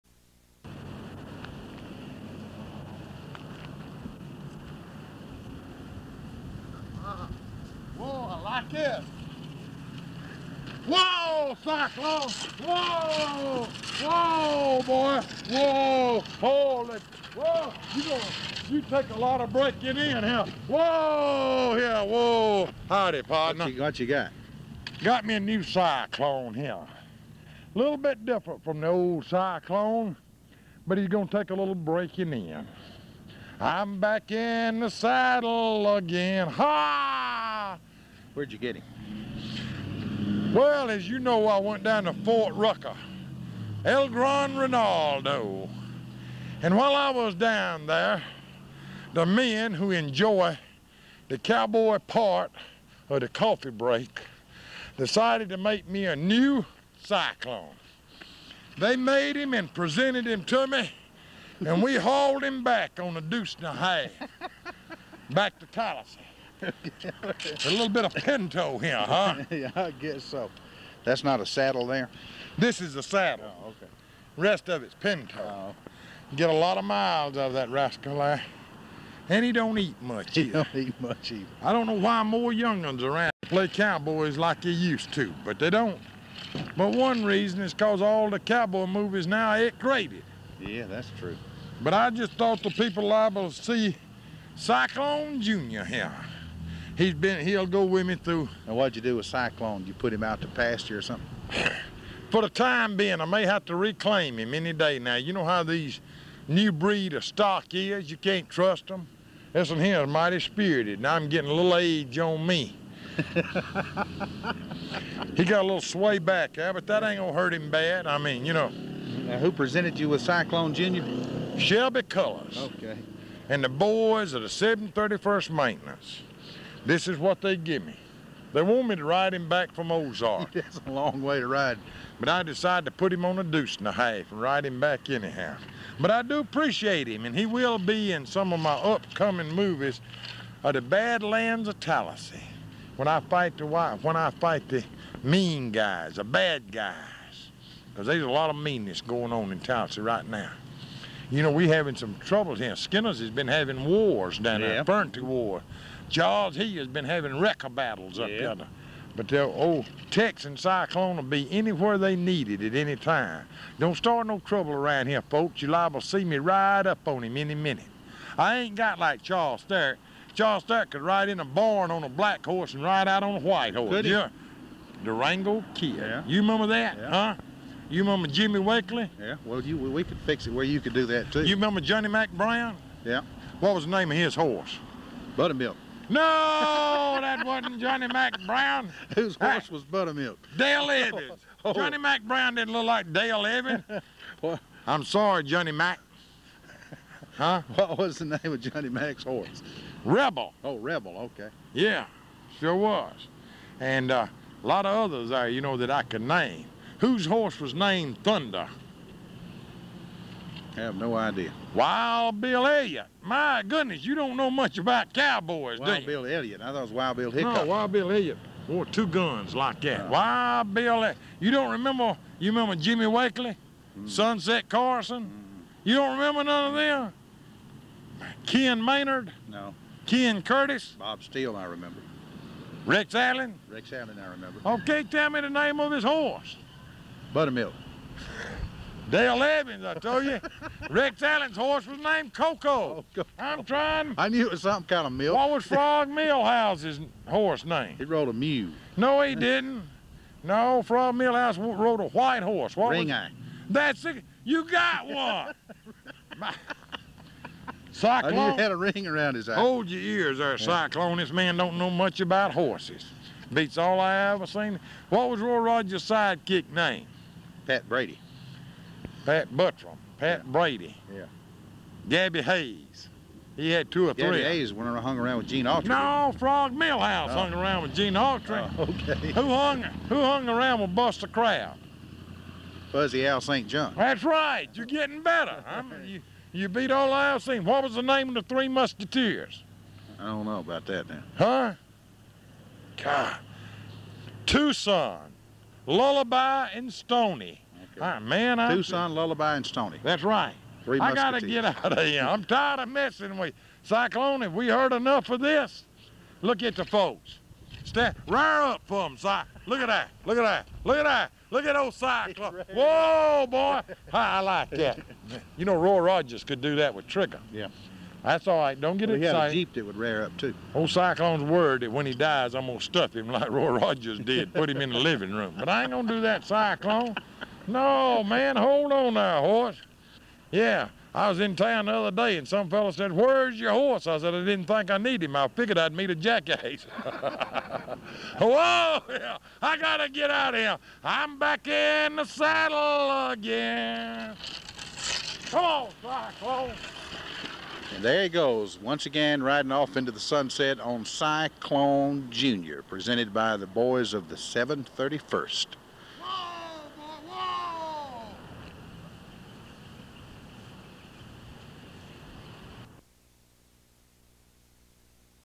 Cyclone Jr. and Cowboys and Horses
Coffee Break production of Ronald Brantley and Jackson Mitchell introducing Cyclone and discussing cowboys and their horses.